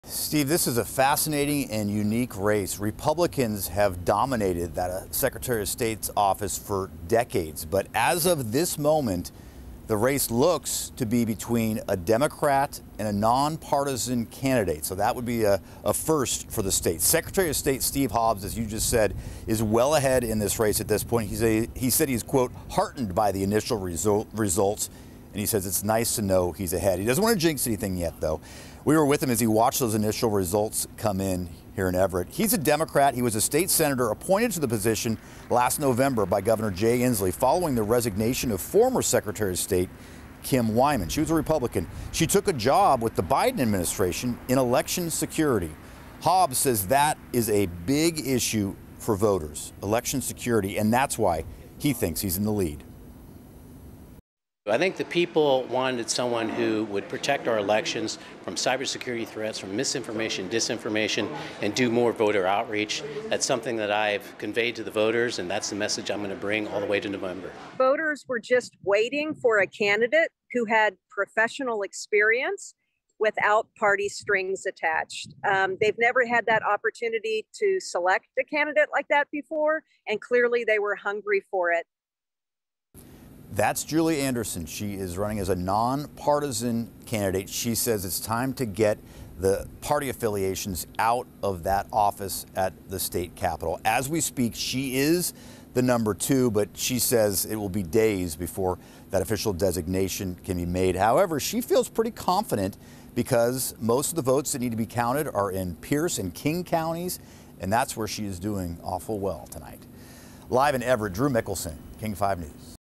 Sen. Steve Hobbs leading in race for Washington Secretary of State
Sen. Steve Hobbs is attempting to hang on to the office for the remaining two years of former Republican Secretary of State Kim Wyman’s four-year term.